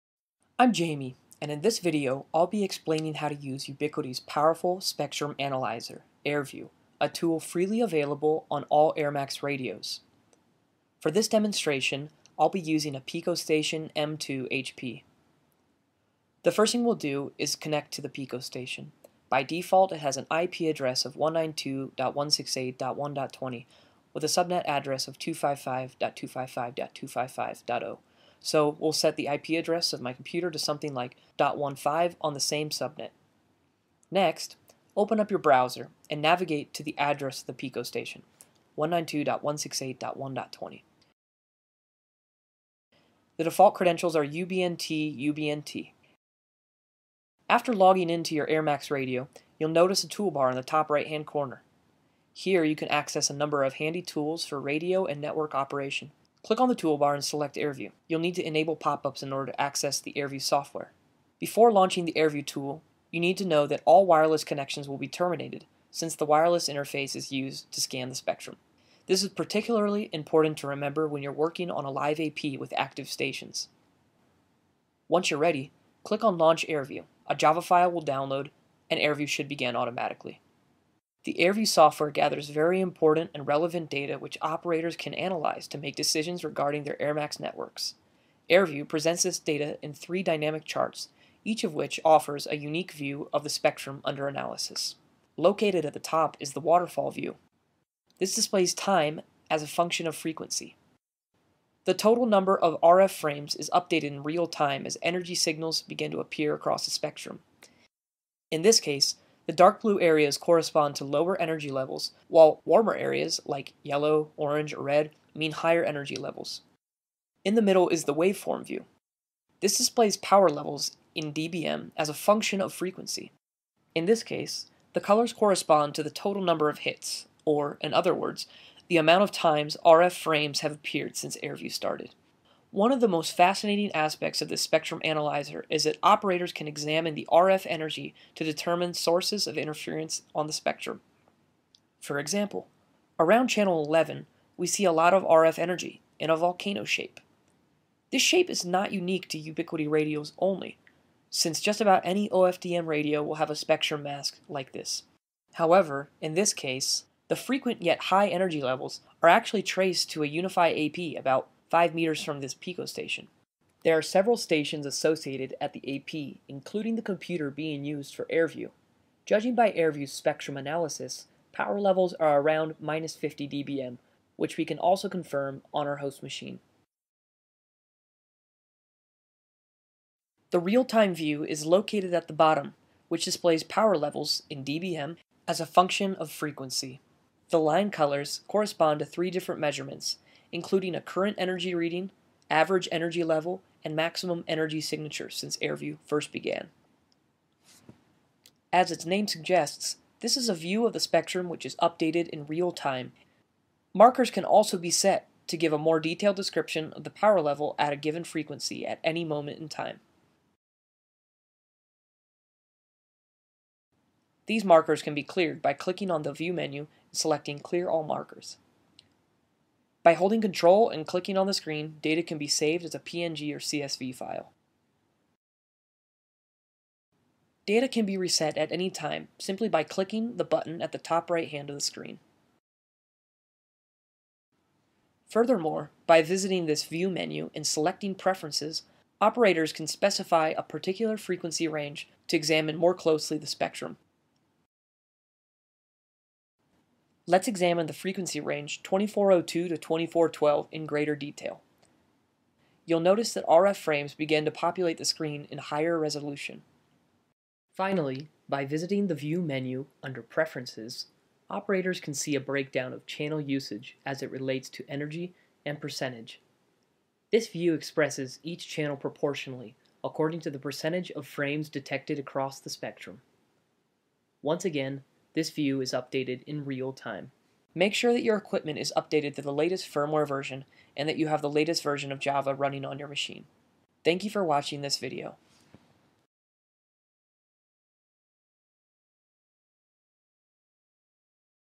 Ubiquiti airView Walkthrough
A clear and concise walkthrough of Ubiquiti's spectrum analyzer, airView, freely available on all M-series devices (airMAX).